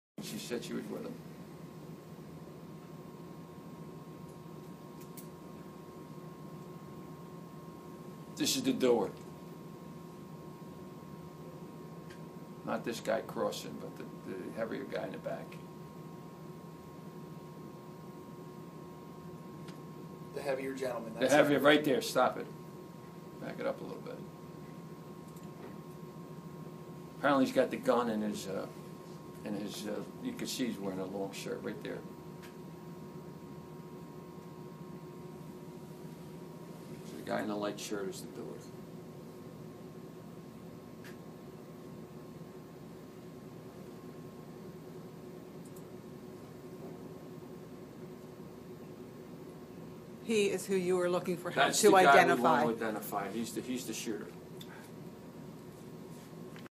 Upper Darby cops looking to ID heavyset male suspect in shooting that occurred early Sunday in area
Upper Darby cops looking to ID heavyset male suspect in shooting that occurred early Sunday in area Kent & Marshall.

By: Rose Quinn - Police reporter
Published on: July 18, 2016